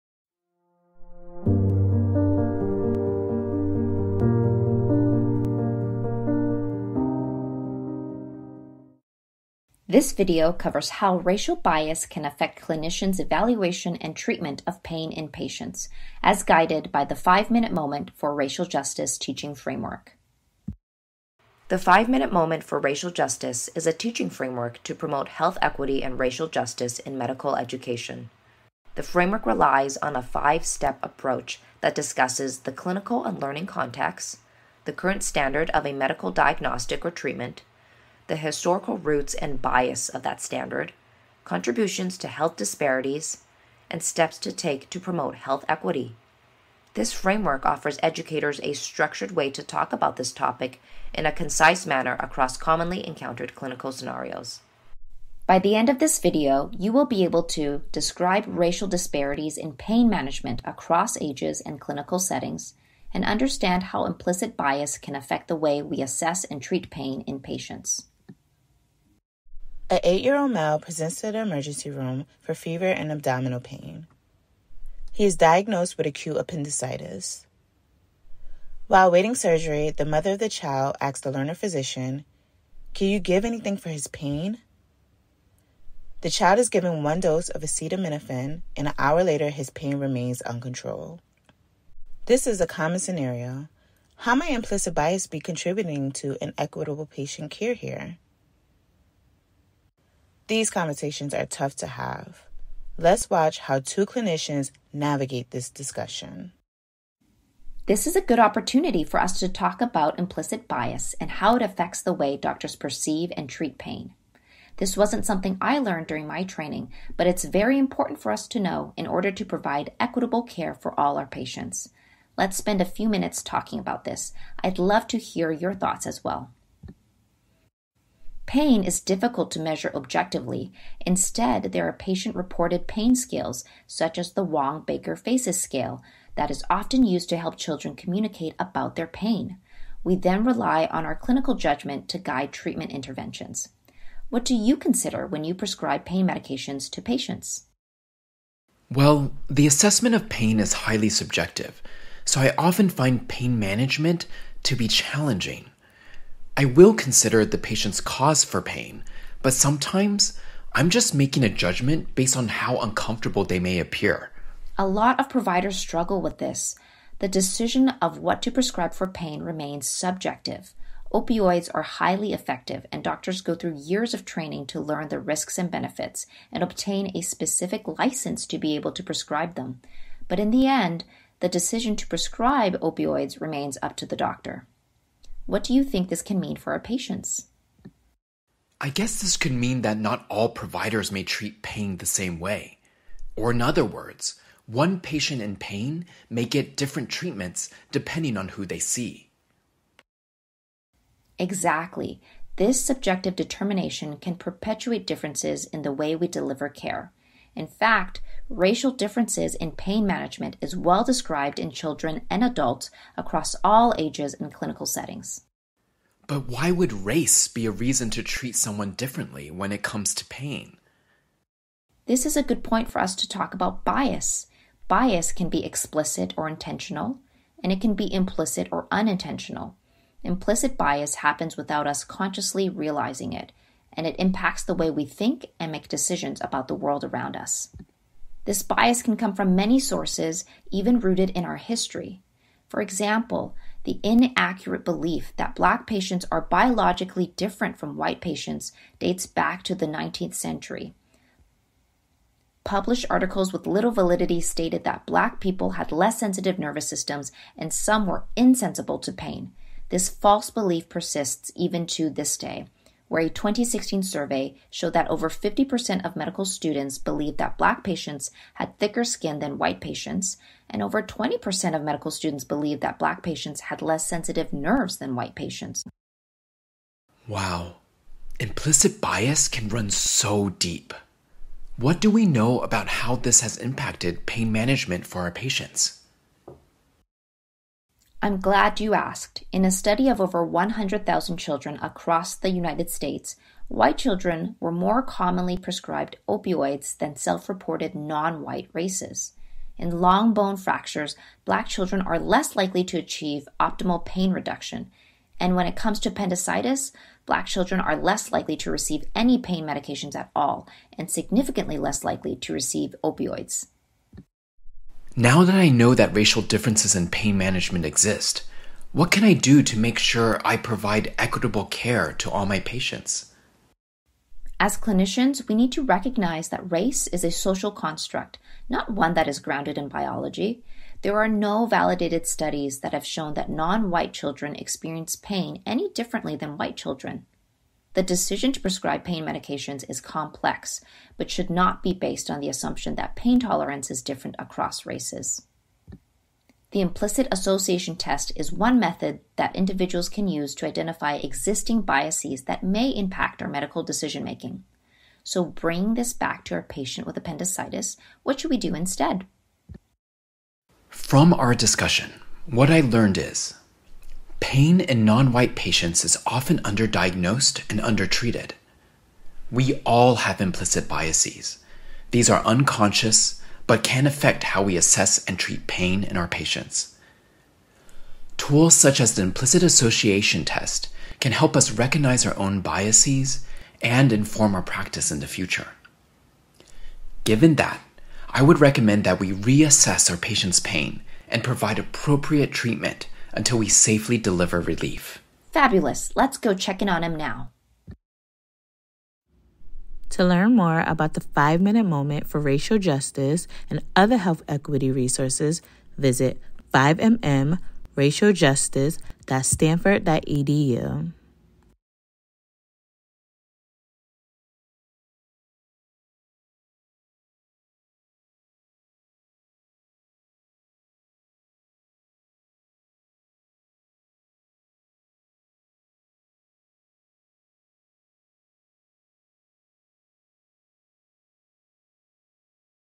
Racial Disparities in Pain Management
A Continuing Medical Education activity presented by the Stanford University School of Medicine.

View the entire 5 Minute Moment for Racial Justice in Healthcare Series